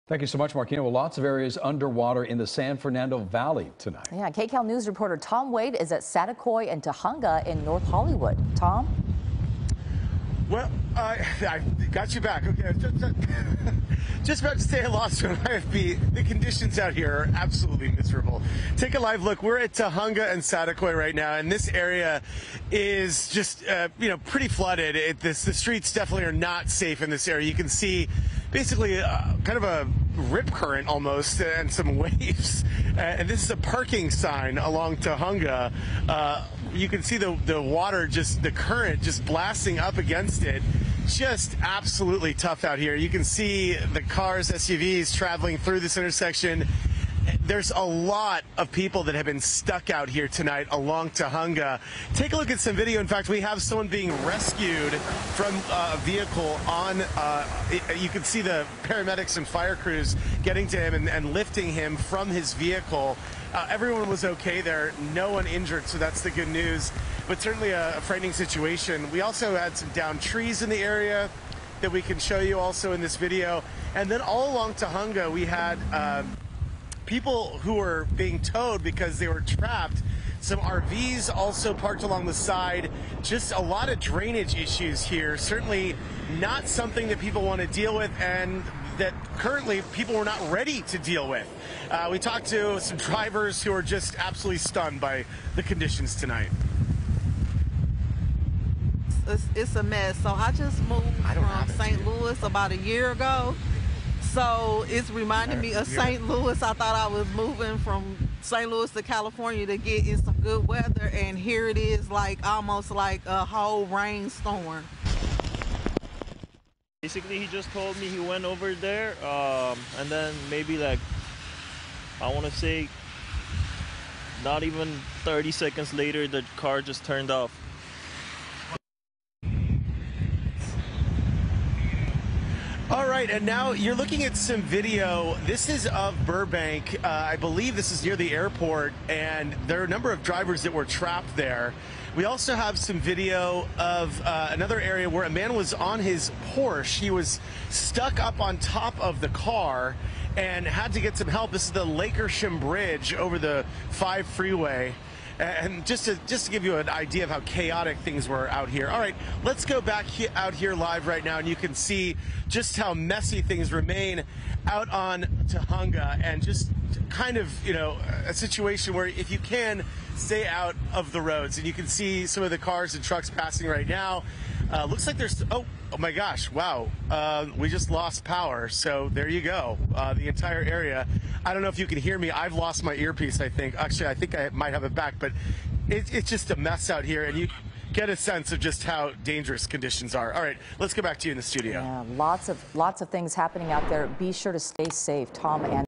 San Fernando Valley flooding
Tom Wait was in North Hollywood where the streets were flooding during a powerful storm.